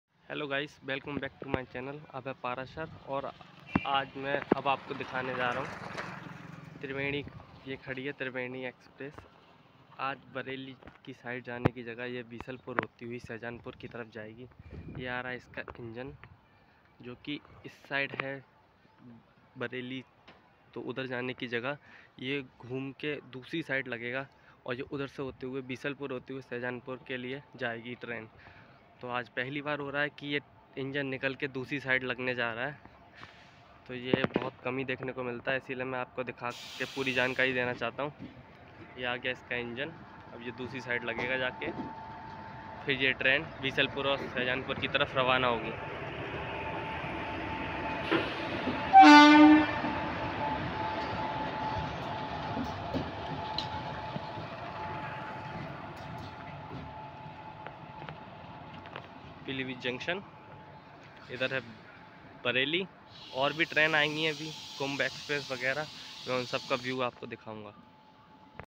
triveni express / pilibhit to shahjahanpur route
aaj phli bar triveni express bisalpur hoti hui shahjahanpur jayegi☺